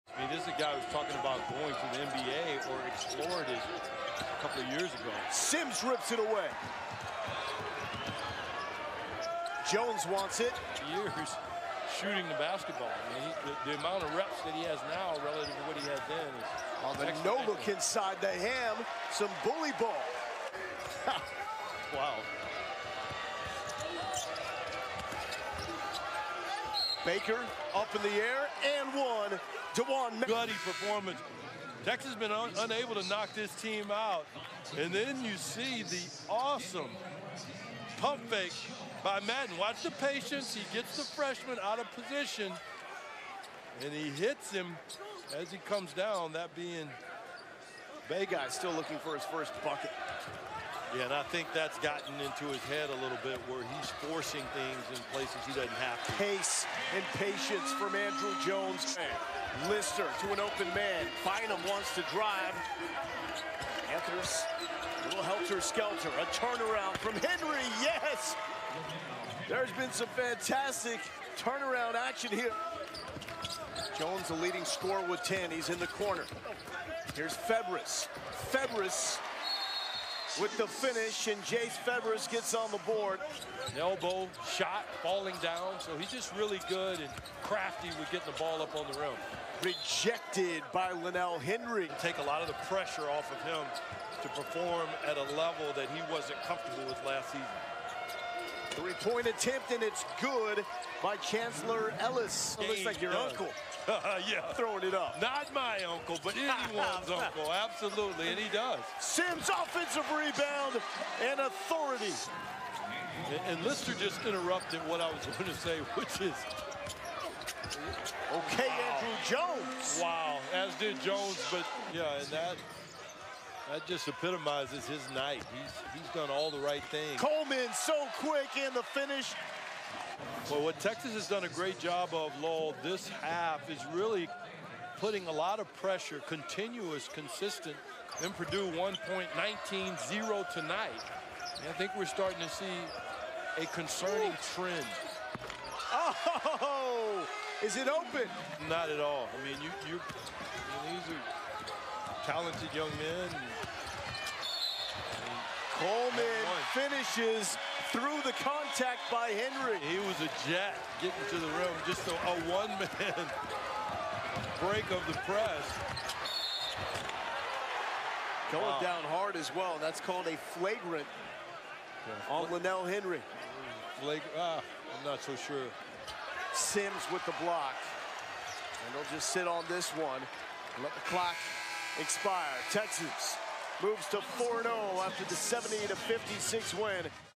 Prairie View A&M vs. Texas Basketball Highlights (2019-20) | Stadium
Texas pulled away in the second half against Prairie View A&M, as the Longhorns picked up a 70-56 win in Austin. Catch all the highlights from the non-conference win for UT right here.